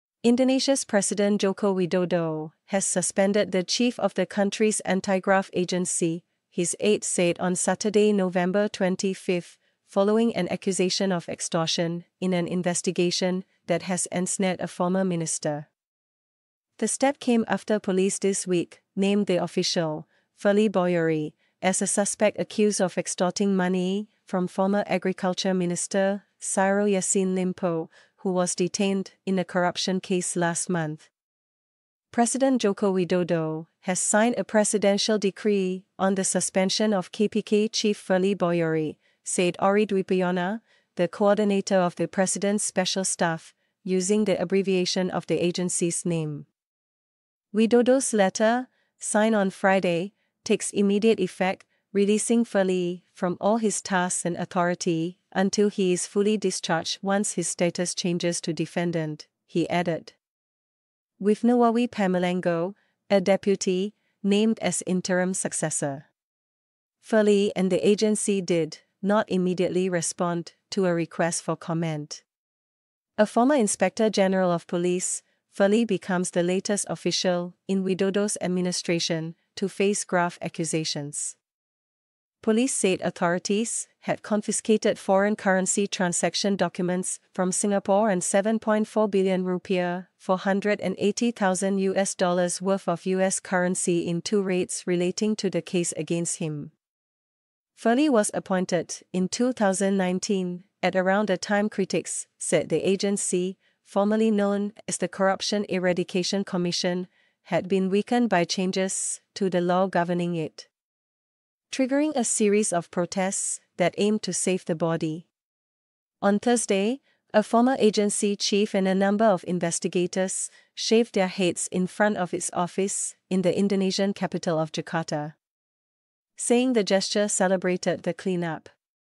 Indonesia suspends anti-graft chief amid extortion probe
The Indonesian anti-corruption agency, known as the KPK, has been at the forefront of the country's efforts to tackle corruption. However, recent events have cast doubt on the agency's ability to maintain its integrity. In this video, we'll be discussing the suspension of the KPK's chief, Firli Bahuri, amid an extortion probe. We'll explore the implications of this move for Indonesia's fight against corruption and what it means for the future of the KPK..
1. Indonesia
2. Anti-graft
3. Chief
4. Suspension
5. Extortion
6. Probe.

💗📌 Singapore